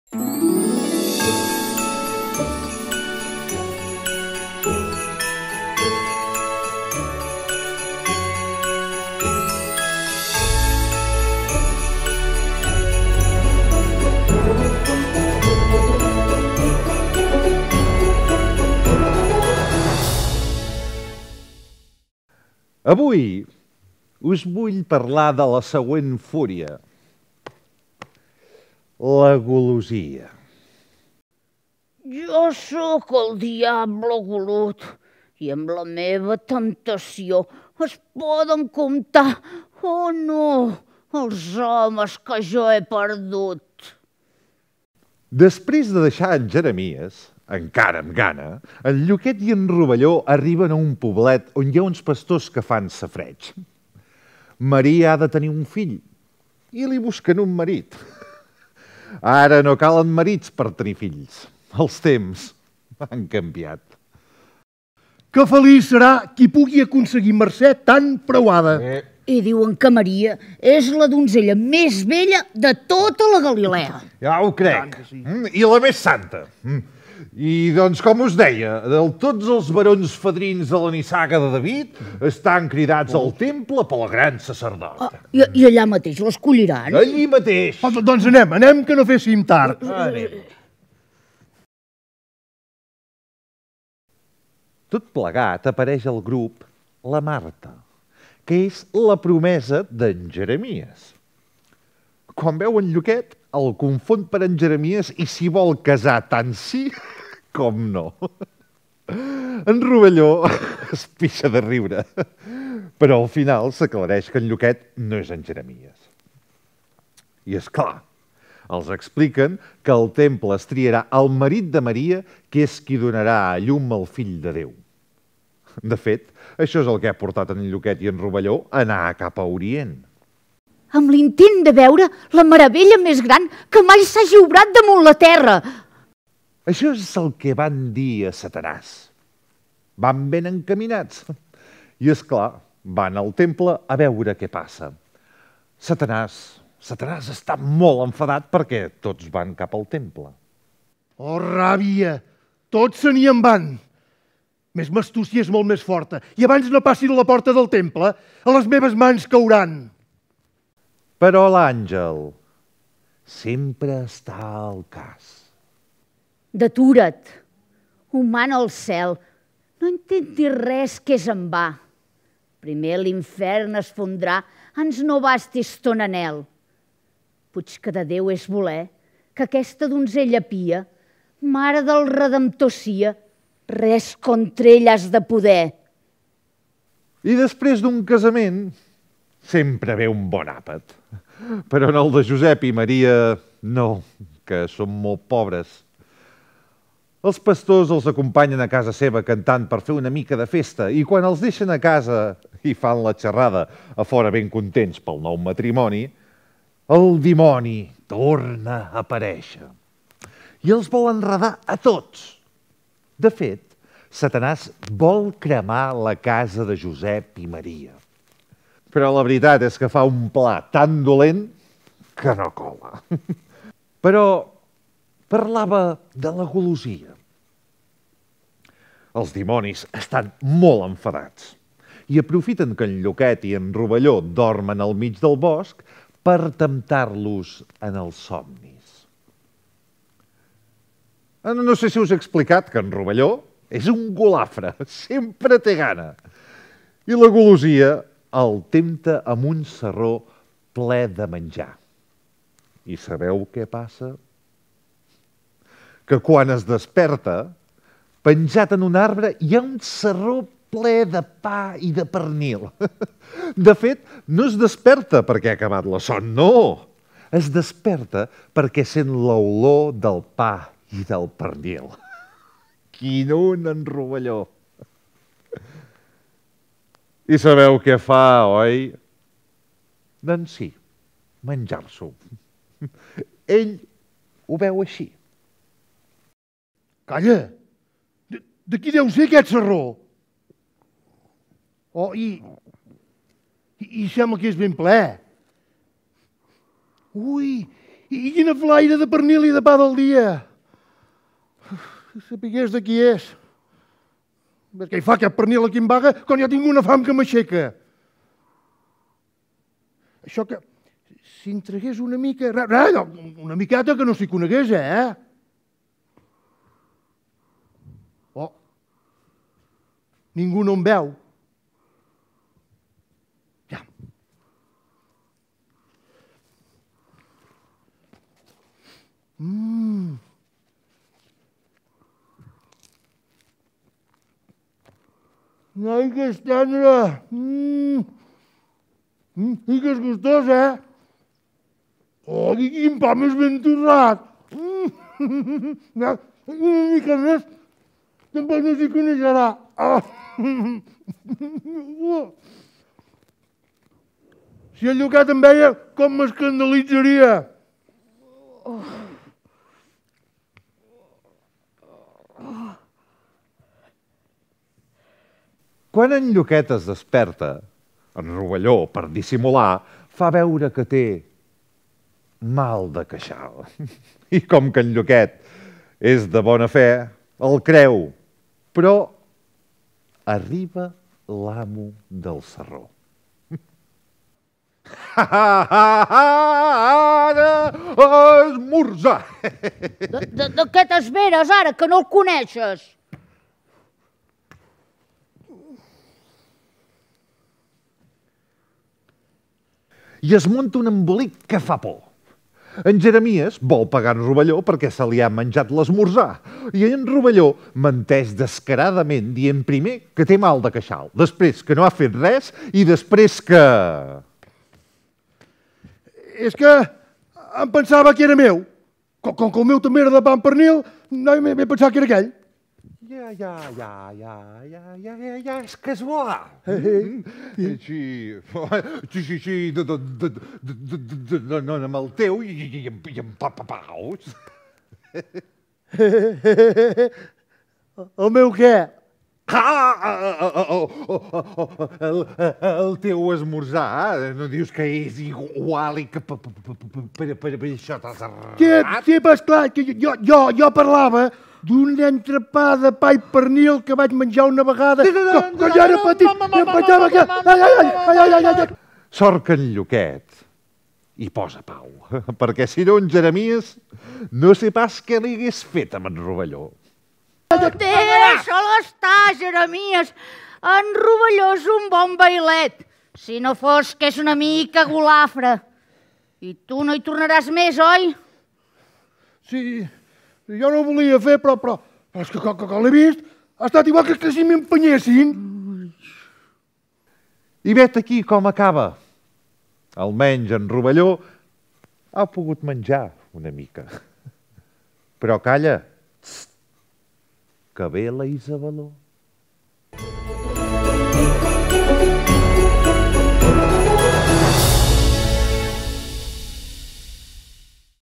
7 Píndoles Capitals - Episodi 3
Al Grup de Teatre de Sant Hipòlit continuem el viatge dels nostres particulars Pastorets explicant-vos les aventures d'en Lluquet i en Rovelló amb 7 Píndoles Capitals. Avui la golosia.

Producció: Grup de Teatre de Sant Hipòlit
Edició vídeo: Gemma Illa
Càmeres: Gemma Illa / Marc Sellés / Esperit Sant
Il·lustracions i Disseny Gràfic: Ana Beatriz Reina